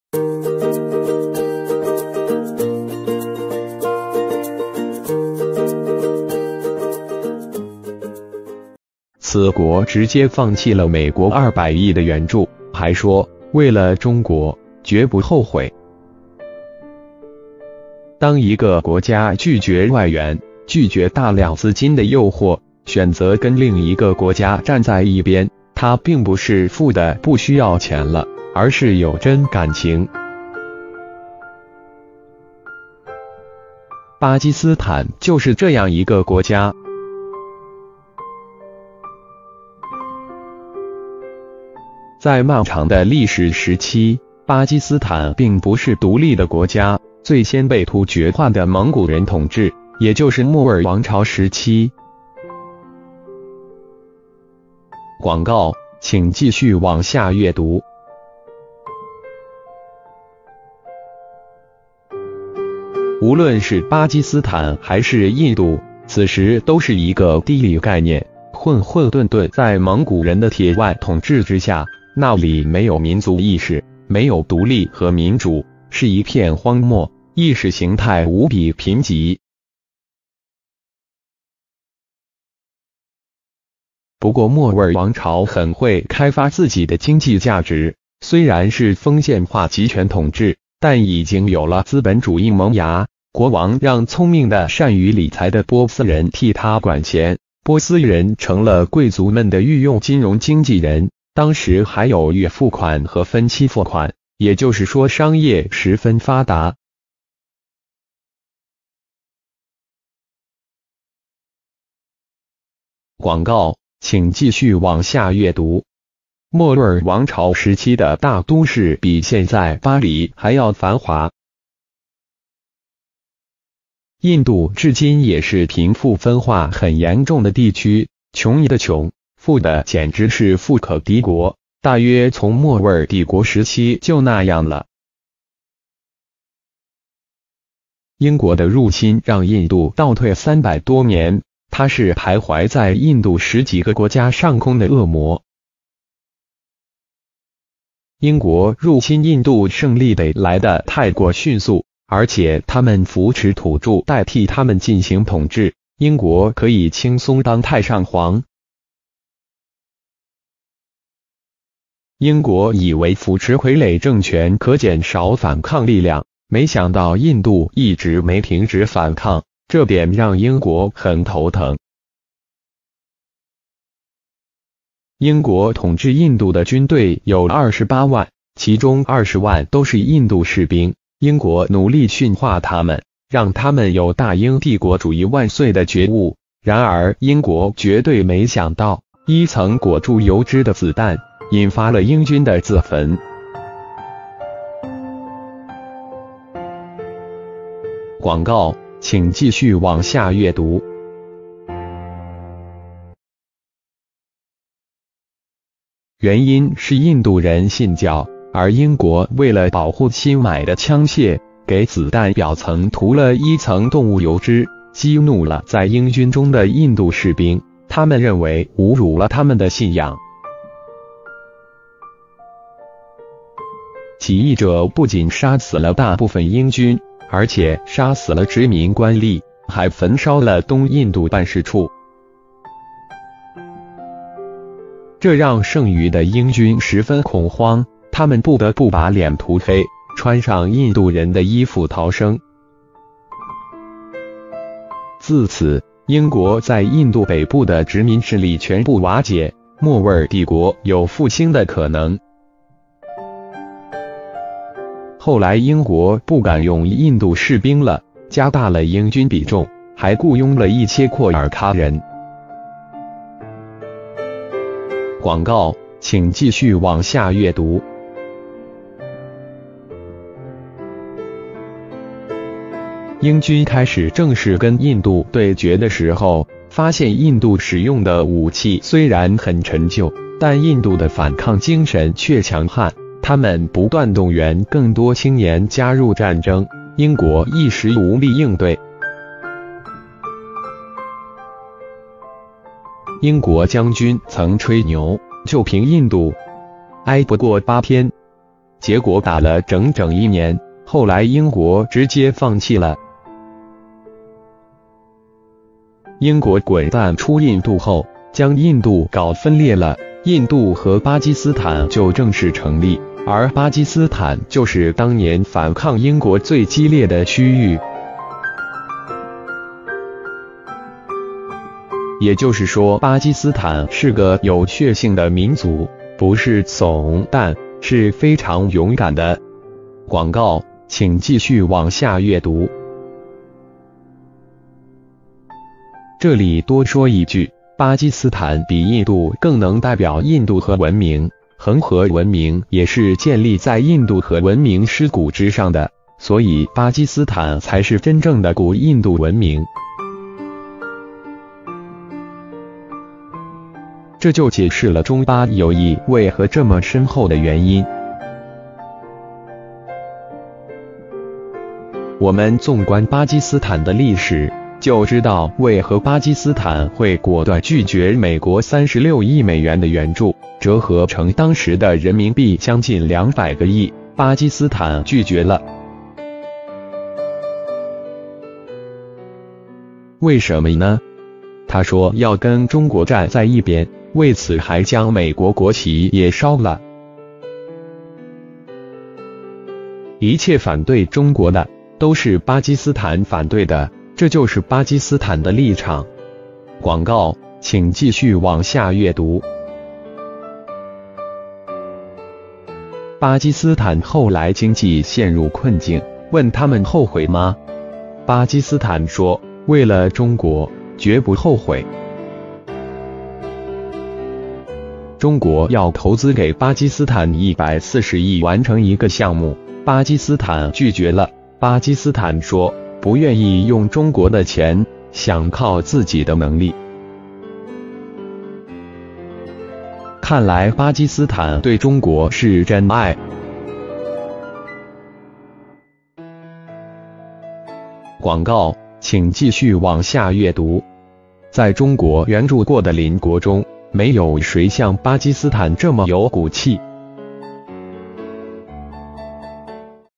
此國直接放棄了美國200億的援助，還說：為了中國，絕不後悔
當一個國家拒絕外援，拒絕大量資金的誘惑，選擇跟另一個國家站在一邊，它並不是富得不需要錢了，而是有真感情。巴基斯坦就是這樣一個國家。在漫長的歷史時期，巴基斯坦並不是獨立的國家，最先被突厥化的蒙古人統治，也就是莫臥兒王朝時期。廣告－請繼續往下閱讀

無論是巴基斯坦還是印度，此時都是一個地理概念，混混沌沌在蒙古人的鐵腕統治之下，那裡沒有民族意識，沒有獨立和民主，是一片荒漠，意識形態無比貧瘠。不過莫臥兒王朝很會開發自己的經濟價值，雖然是封建化集權統治，但已經有了資本主義萌芽，國王讓聰明的善於理財的波斯人替他管錢，波斯人成了貴族們的御用金融經紀人，當時還有預付款和分期付款，也就是說商業十分發達。廣告－請繼續往下閱讀

莫臥兒王朝時期的大都市比現在巴黎還要繁華。印度至今也是貧富分化很嚴重的地區，窮的窮，富的簡直是富可敵國，大約從莫臥兒帝國時期就那樣了。英國的入侵讓印度倒退三百多年，它是徘徊在印度十幾個國家上空的惡魔。英國入侵印度勝利得來得太過迅速，而且他們扶持土著代替他們進行統治，英國可以輕鬆當太上皇。英國以為扶持傀儡政權可減少反抗力量，沒想到印度一直沒停止反抗，這